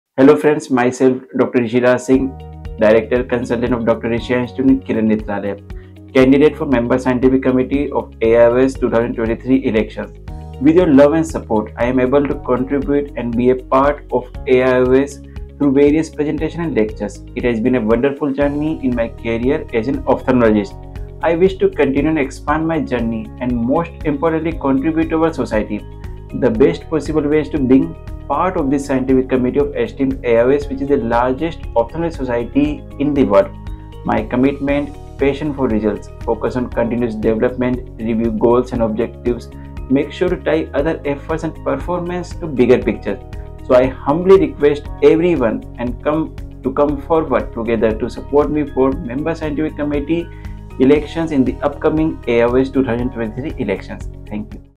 Dr. Rishi, your candidate for the AIOS Scientific Committee 2023!
Join me on a journey of innovation and excellence in ophthalmology - Vote for Dr. Rishi, your candidate for the AIOS Scientific Committee 2023!
📍Address:-1363/64,Sector-6 Karnal-132001